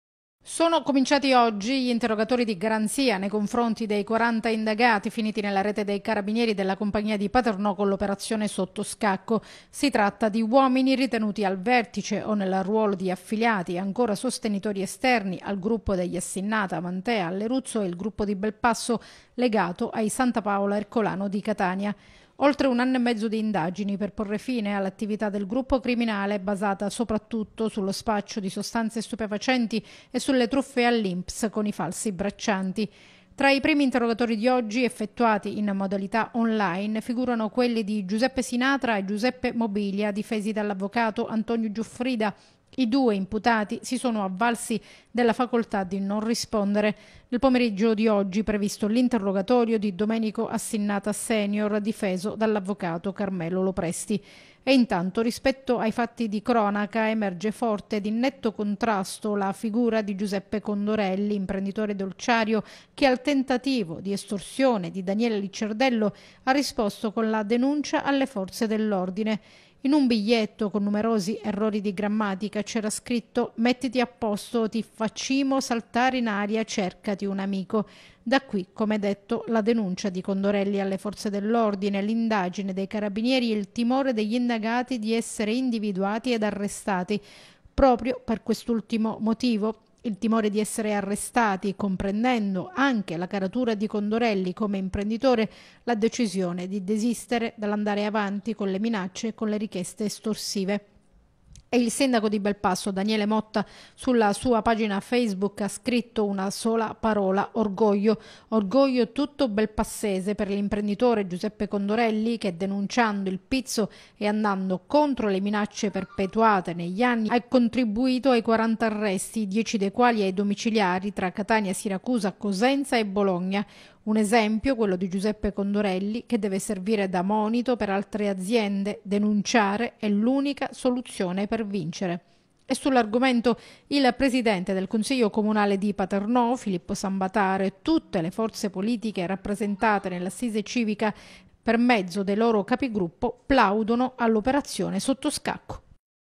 “SOTTO SCACCO”, AL VIA GLI INTERROGATORI DI GARANZIA
AL VIA GLI INTERROGATORI DI GARANZIA FRUTTO DELL’OPERAZIONE “SOTTO SCACCO”, CONDOTTA A PATERNO’, DAI CARABINIERI DELLA LOCALE COMPAGNIA.